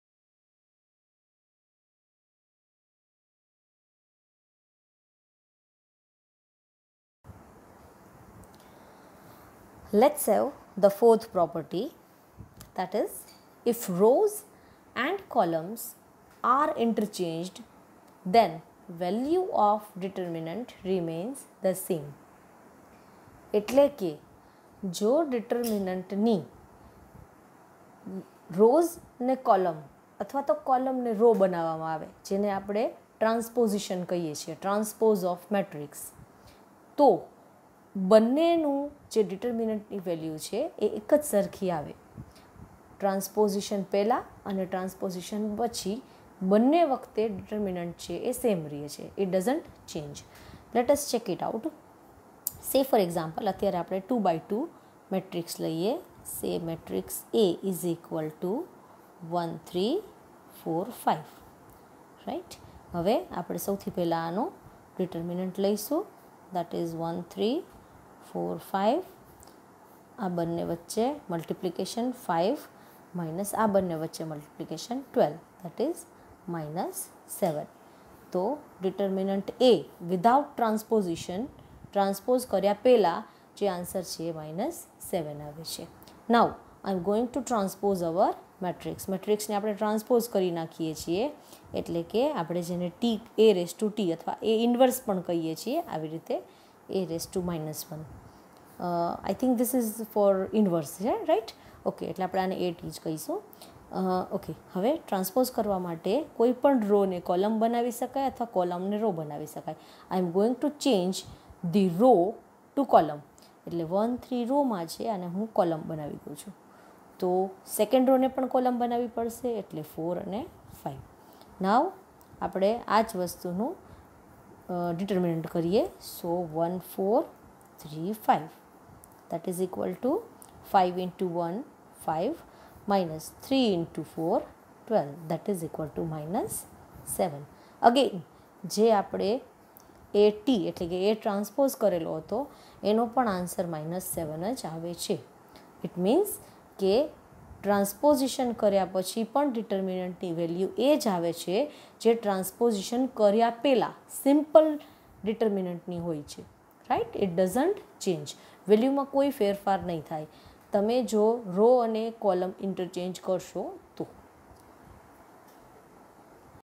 Determinant | Chapter 1 | Property Of Determinant | Value of Determinant is equal to it's Transpose
This video contains one of the properties of determinant. It says "If rows and columns are interchanged, then the value remains the same". This video contains very easy approach to understand and prove this property of determinant with given example.